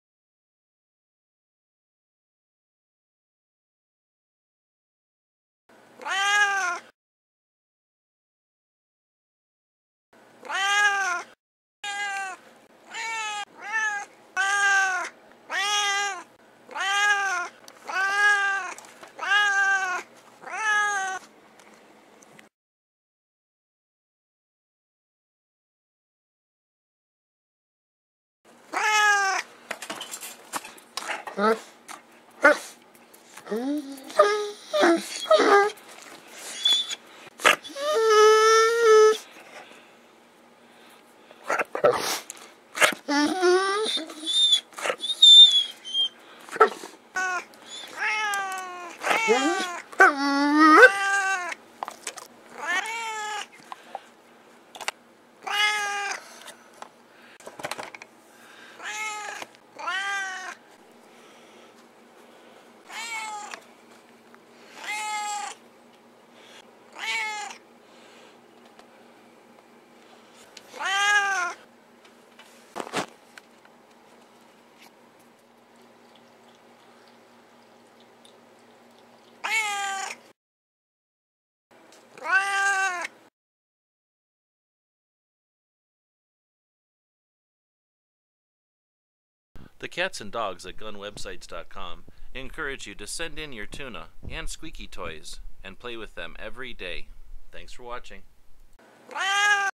No more gun videos ...
We are under new management, and will no longer post any gun videos